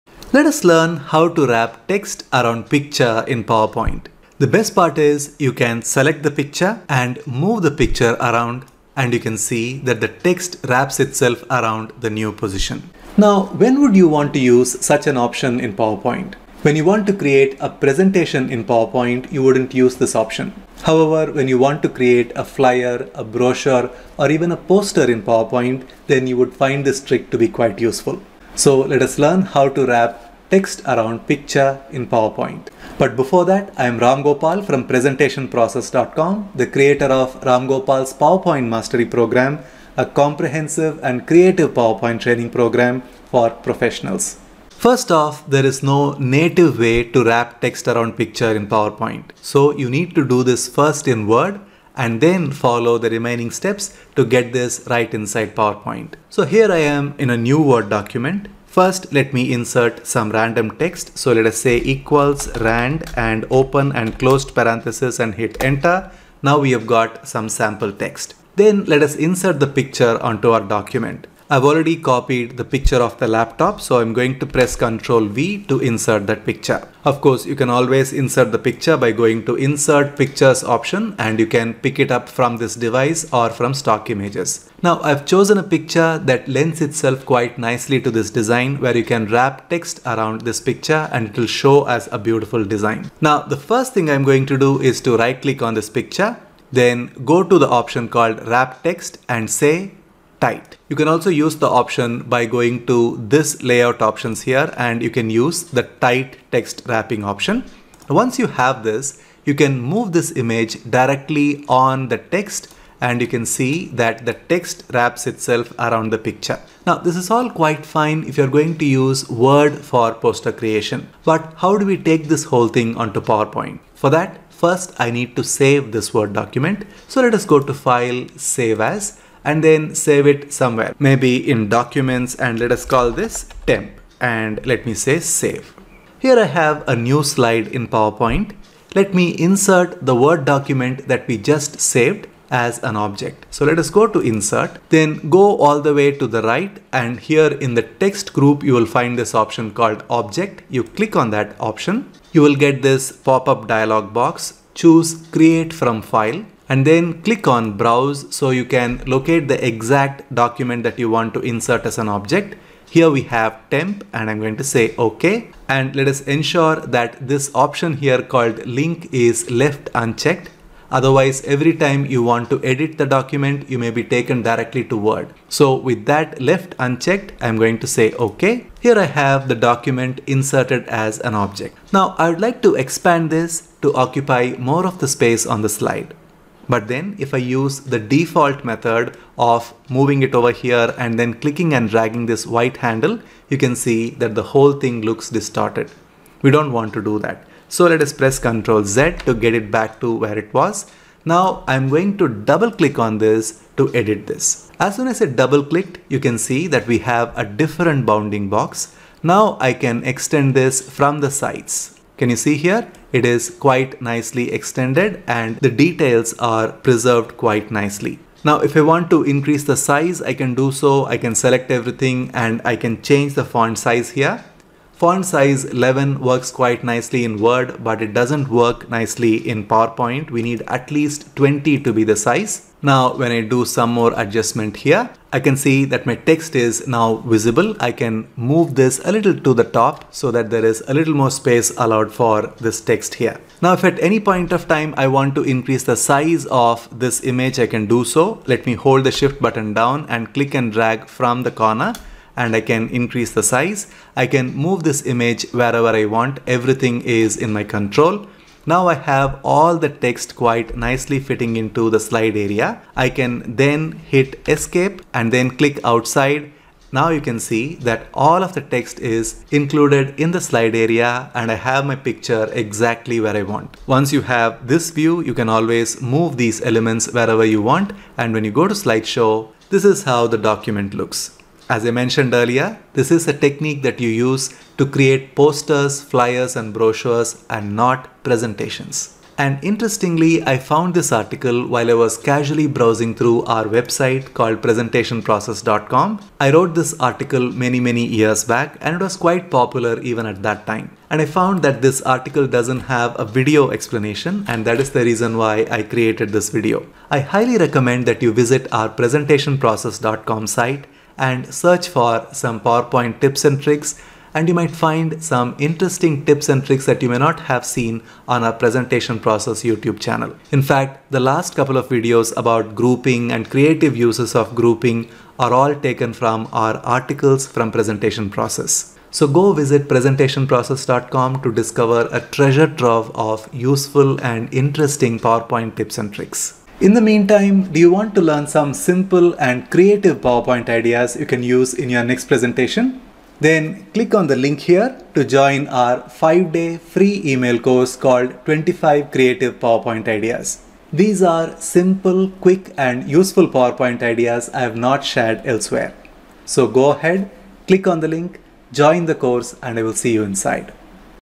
Wrap text around Picture in PowerPoint
Learn how to Wrap text around Picture in PowerPoint. You can use this option a flyer, brochure or poster in PowerPoint.  *Ramgopal's PowerPoint Mastery TRAINING Program*
Links and Resources:
Comprehensive All In One PowerPoint TEMPLATES Bundle 2.0